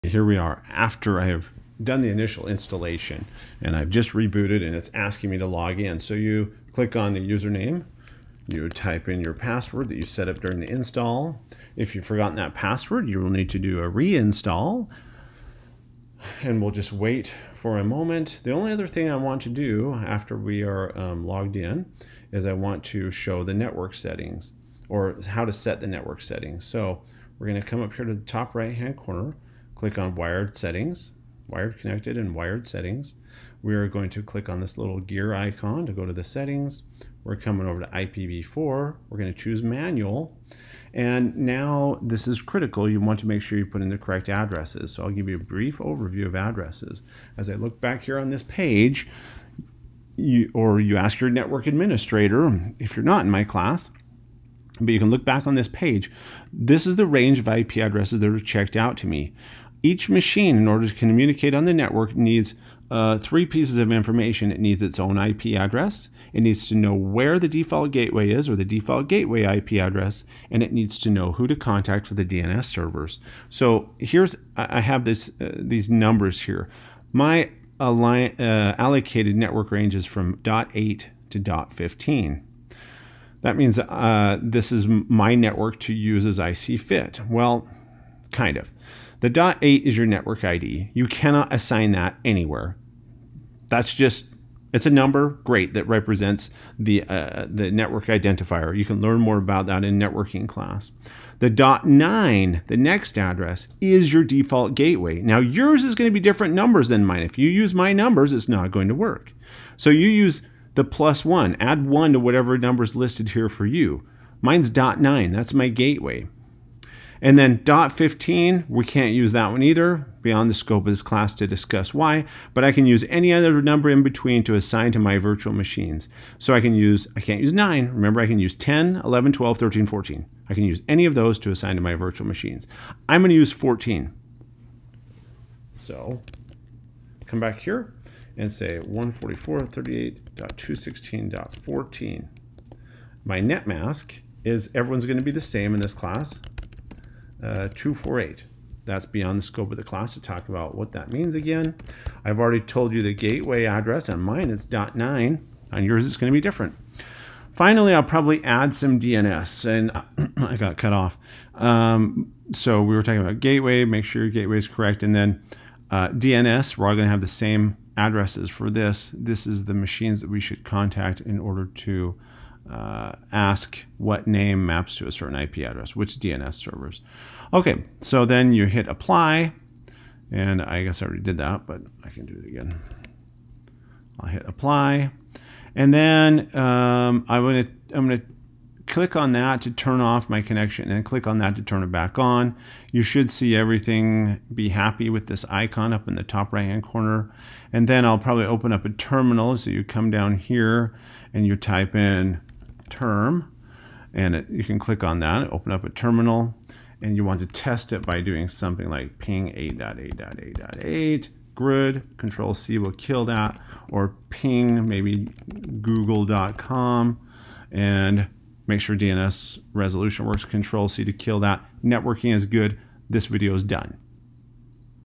Linux: Ubuntu Focal GUI network settings
How to set your network settings in Ubuntu focal fossa